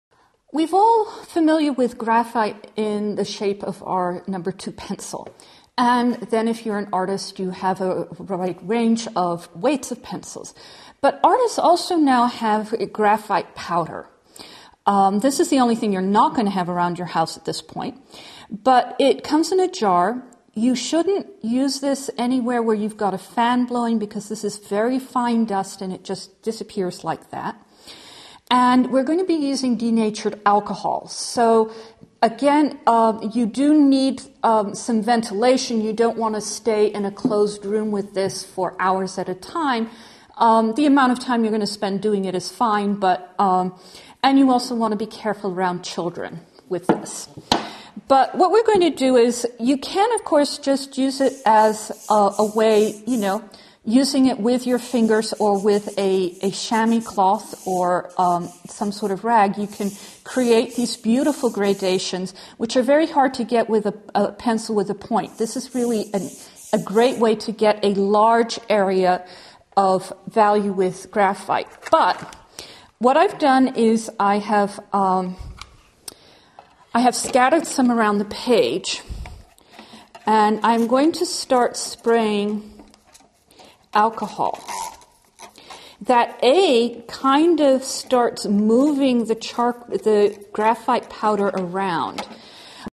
Design Media: Types of Graphite
Design Media: Types of Graphite. Part of the series: Playing With Design Media. Types of graphite for design include pencil and powder forms. Use different types of graphite with tips from an artist in this free design video.